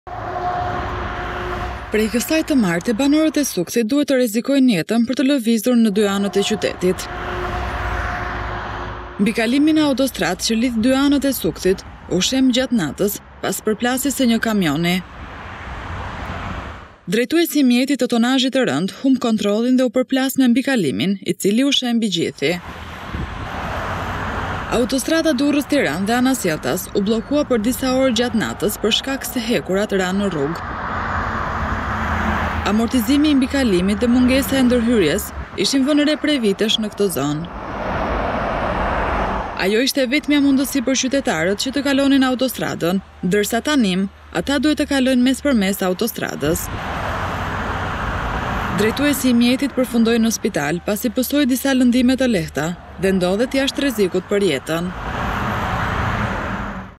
Kamioni shembi mbikalimin/ Banorët në Sukth nuk kanë mundësi kalimi në anën tjetër të qytetit
Mbikalimi i këmbësorëve në autostradën Durrës-Tiranë, pranë Sukthit u shemb pasi një kamion u përplas me strukturën. Ngjarja sipas policisë u shënua në orët e para të mëngjesit, pasi drejtuesi ka humbur kontrollin  mbi drejtimin e mjetit dhe kamioni është përplasur me mbikalimin e këmbësorëve. Prej kësaj të marte banorët e Sukthit duhet të rrezikojnë jetën për të lëvizur në dy anët e qytetit.

Mbukalimi në autostradë që lidh dy anë të Sukthit u shemb gjatë natës, pas përplasjes së një kamioni. Drejtuesi i mjetit të tonazhit të rëndë humbi kontrollin dhe u përplas me mbikalimin i cili u shemb i gjithi. Atostrada Durrës-Tiranë dhe anasjelltas u bllokua për disa orë gjatë natës për shkak se hekurat ranë në rrugë.

Amortizimi i mbikalimit dhe mungesa e ndërhyrjes ishin vënë re prej vitesh në zonë. Ajo ishte e vetmja mundësi për qytetarët që të kalonin autostradën, ndërsa tanimë ata duhet të kalojnë mes për mes autostradës.

Drejtuesi i mjetit përfundoi në spital pasi pësoi disa lëndime të lehta dhe ndodhet jashtë rrezikut për jetën.